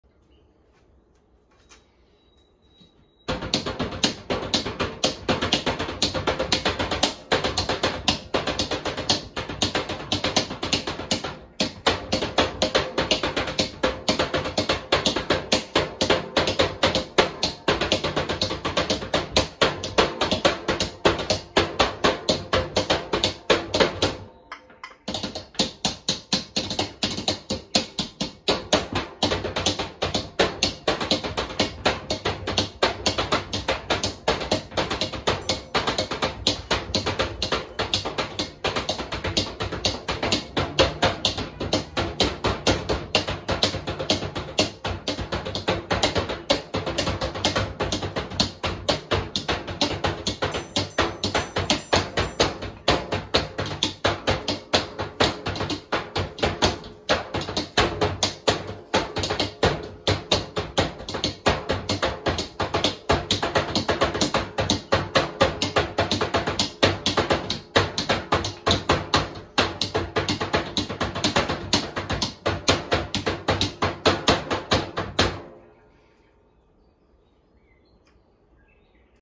Dodi Cockatoo Dances to Trashcan Drums
Yes they really do dance.